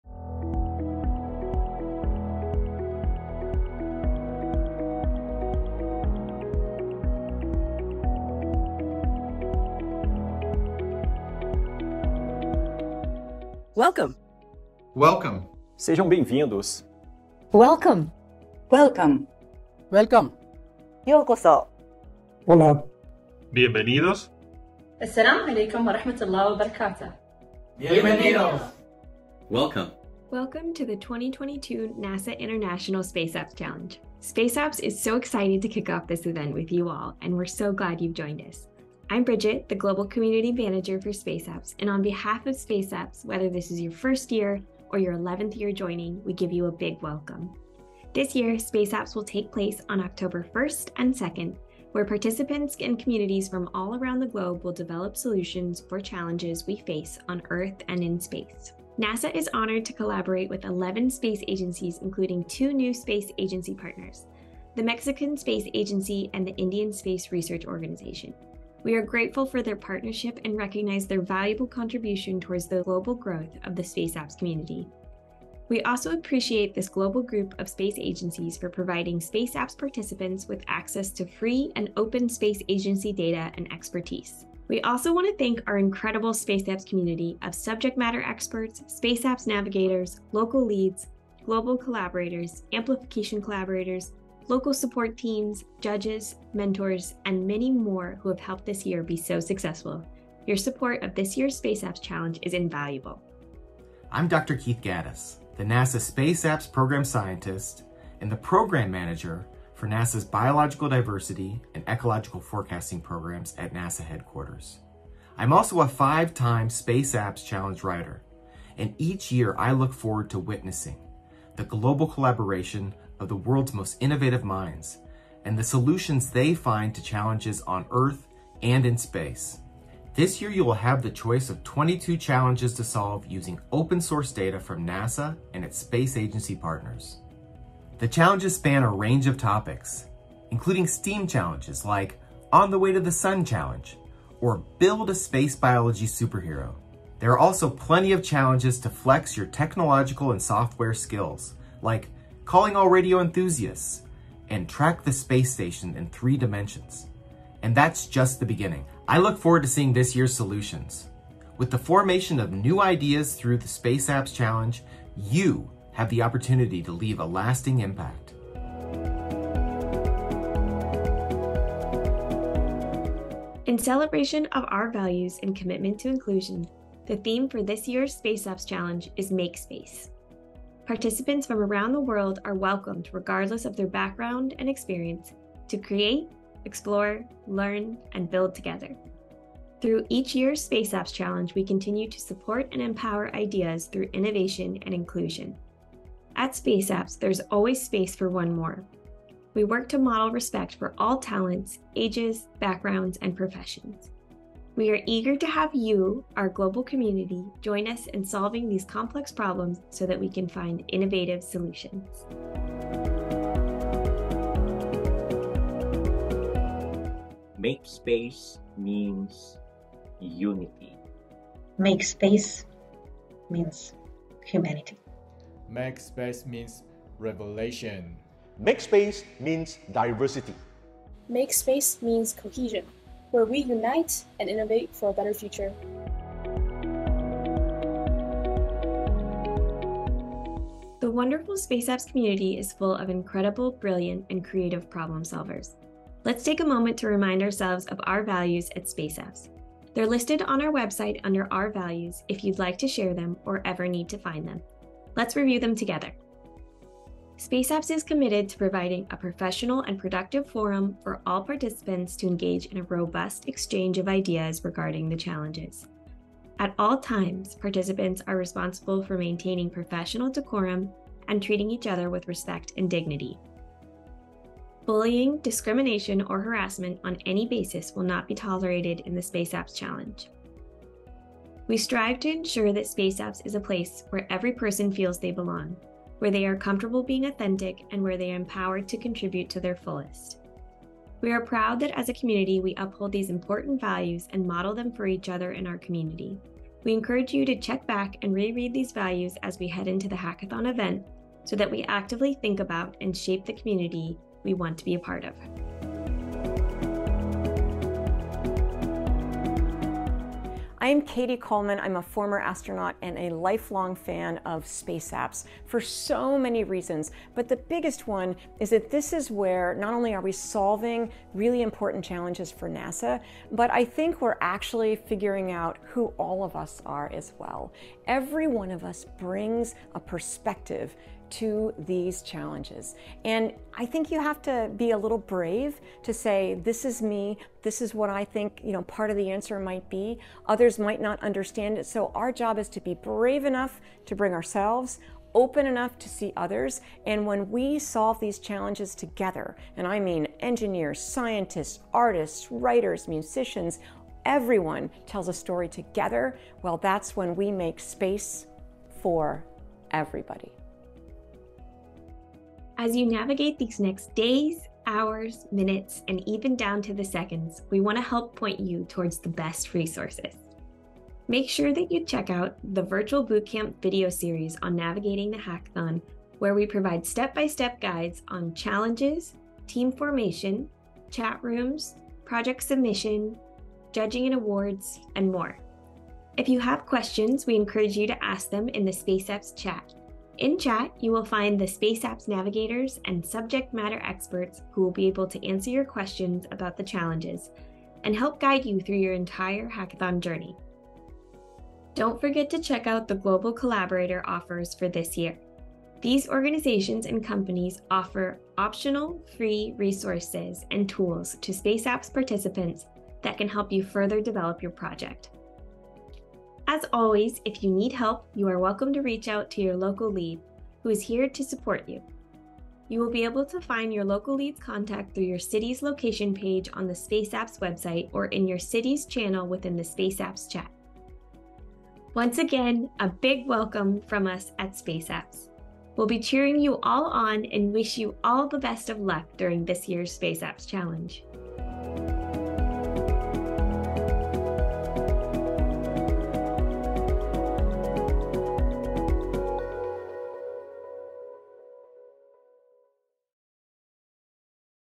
Welcome to Space Apps 2022 | NASA Space Apps Challenge
Welcome to the 2022 NASA International Space Apps Challenge! We are thrilled to kick off this year’s hackathon and MakeSpace with our community.

Hear exclusively from the Space Apps Program Scientist, Dr. Keith Gaddis, former NASA astronaut, Dr. Cady Coleman, and our 11 Space Agency Partners - the Australian Space Agency, Brazilian Space Agency, Canadian Space Agency, ESA (European Space Agency), Indian Space Research Organization, Japan Aerospace Exploration Agency, Mexican Space Agency, National Space Activities Commission of Argentina, National Space Science Agency of Bahrain, Paraguayan Space Agency, and the South African National Space Agency. 🚀

Connect with the SpaceApps community!